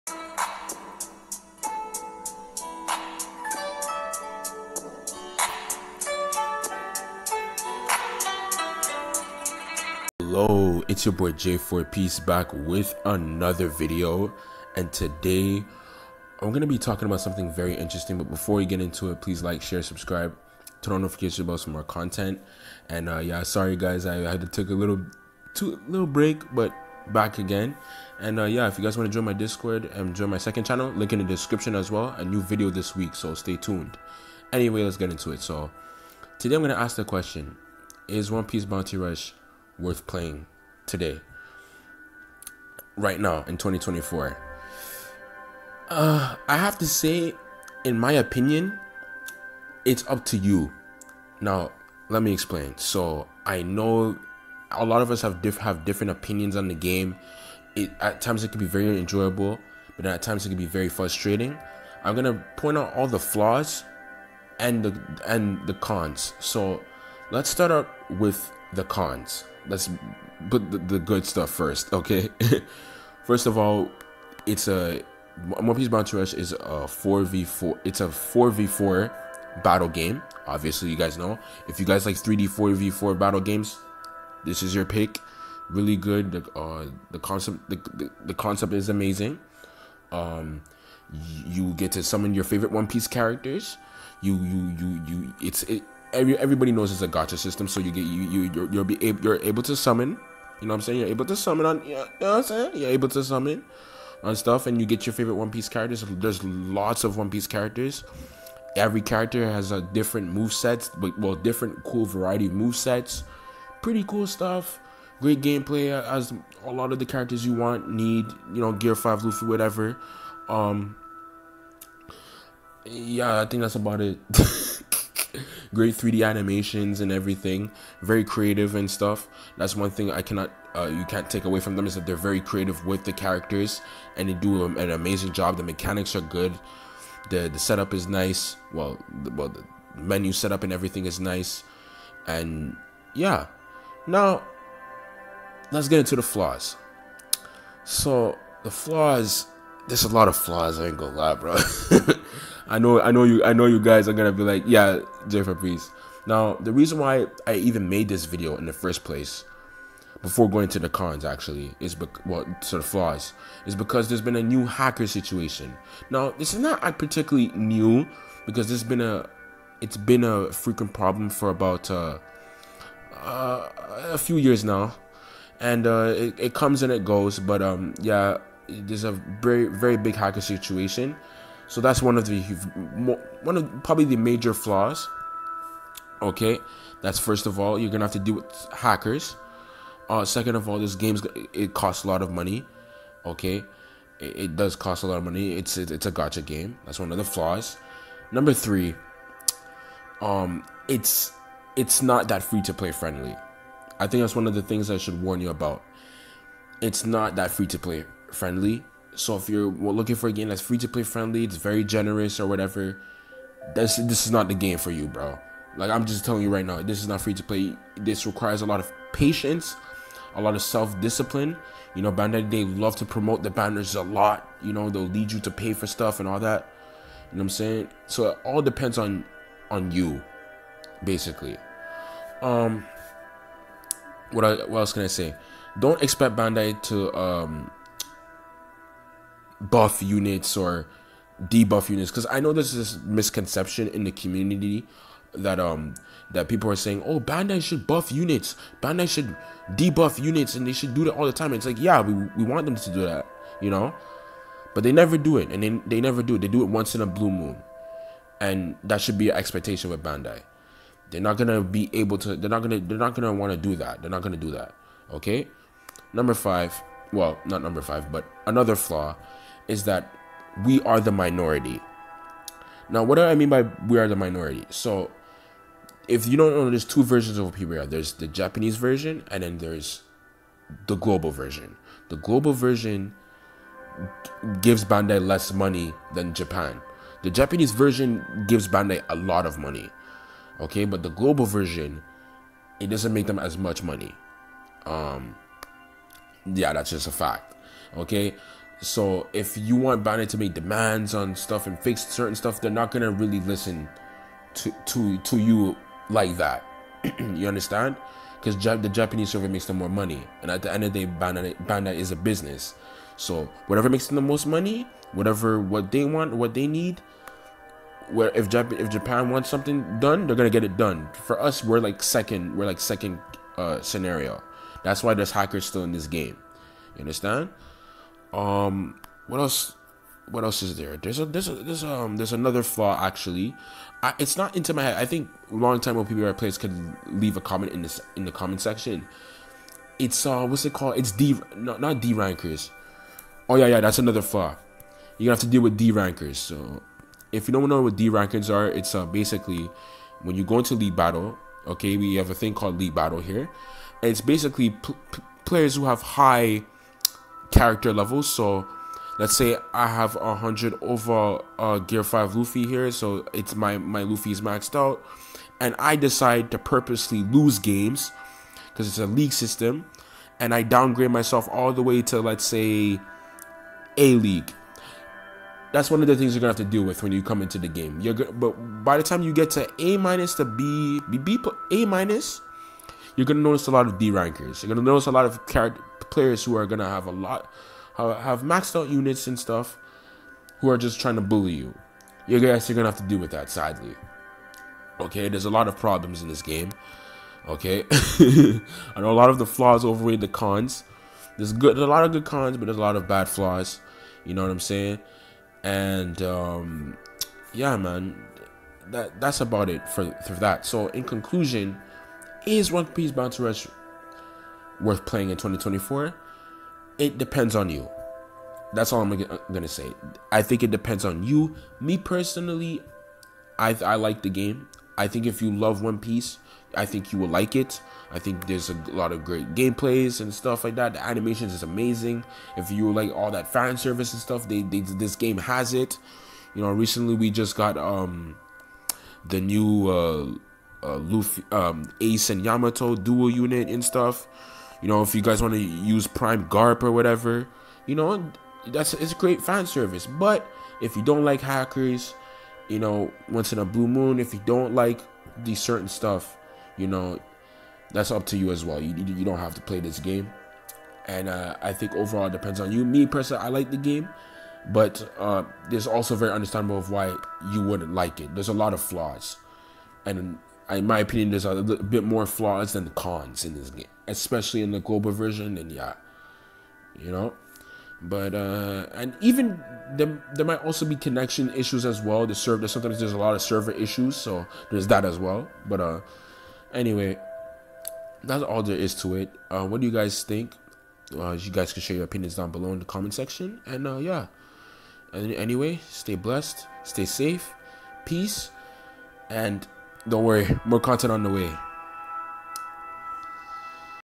IS ONE PIECE BOUNTY RUSH WORTH PLAYING IN 2024?
Hello it's your boy jforpiece and Today I'm going to be talking about if One Piece Bounty Rush is WORTH PLAYING in 2024